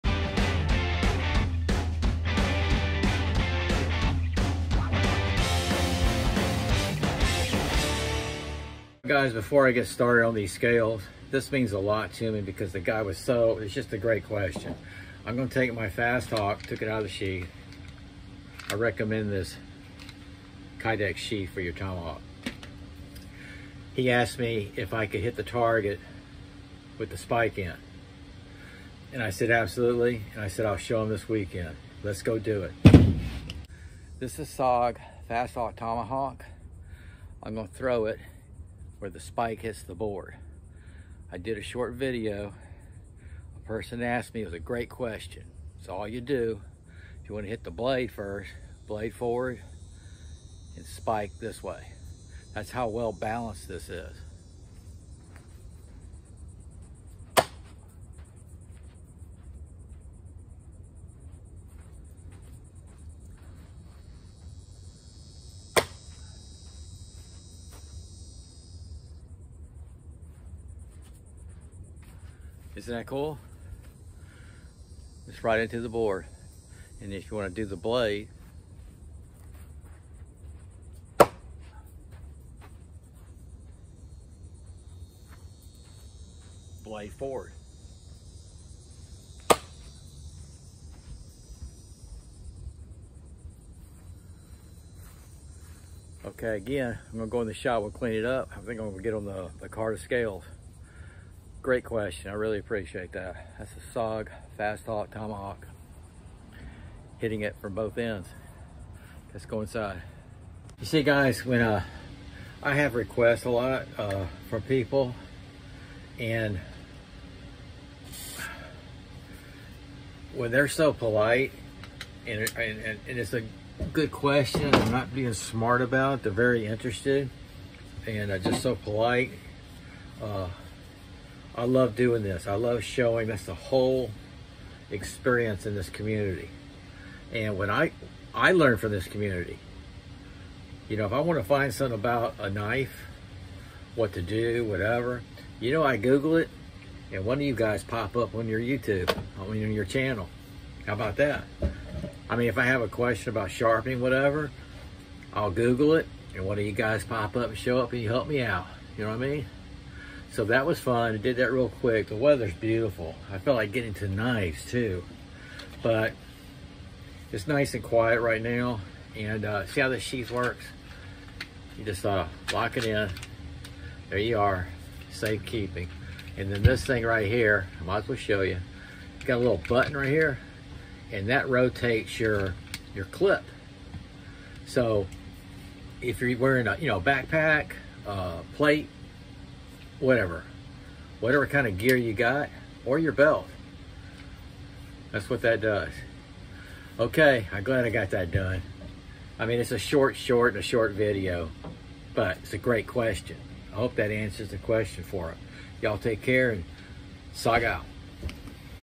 SOG Fasthawk Tomahawk thrown in board spike end.By request
Requested to show hitting the target with the spike end.Excellent question